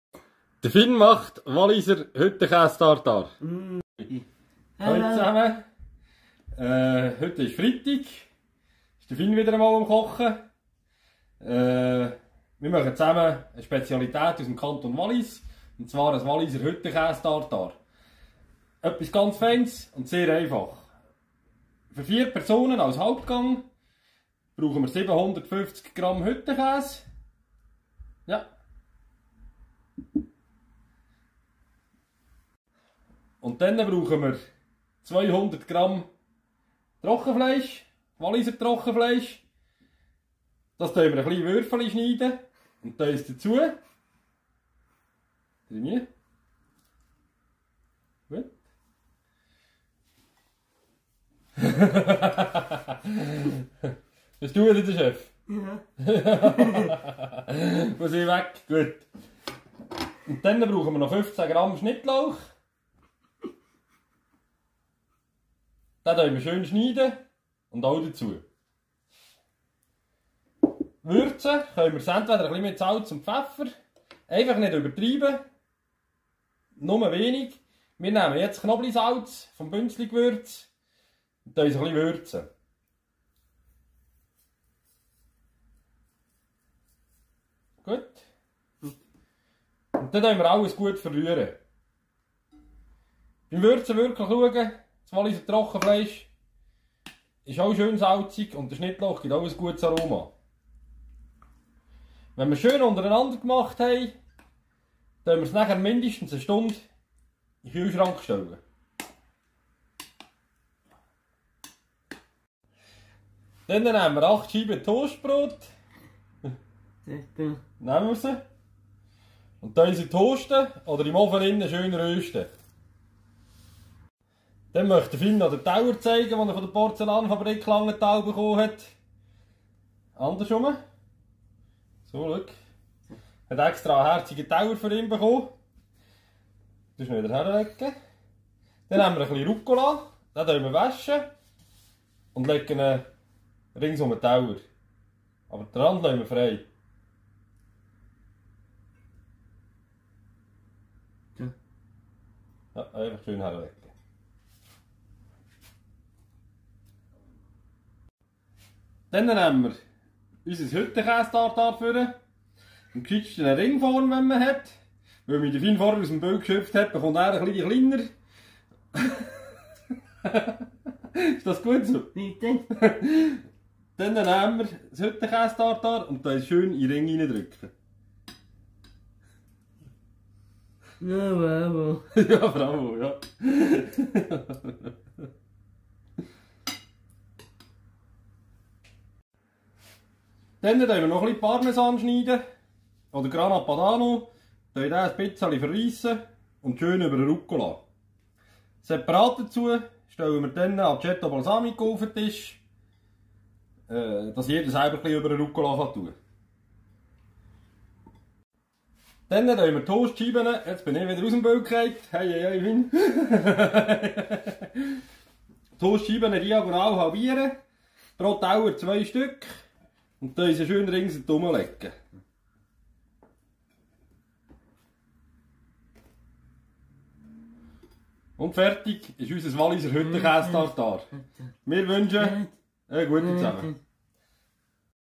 Finn macht Walliser Hüttenkäse-Tatar
Für 4 Personen als Hauptgang oder
8 Personen als Vorspeise

750g Hüttenkäse
200g Walliser Trockenfleisch, gewürfelt
15g Schnittlauch, geschnitten
etwas Ruccola
etwas Parmesan, dünn geschnitten
8 Scheiben Toastbrot
Aceto Balsamico
Salz, Pfeffer oder Knobli Salz von Bünzligwürz

Hüttenkäse, Trockenfleisch und Schnittlauch in einer Schüssel vermischen und mit wenig Knobli Salz oder Salz und Pfeffer würzen.
Mindestens 1 Stunde kühl stellen.
Ruccola waschen, am Tellerrand verteilen und mit etwas Parmesan bestreuen.
Mit hilfe einer runden Form das Tatar in der Tellermitte plazieren.
Toastbrot leicht toasten, diagonal halbieren und auf dem Teller verteilen.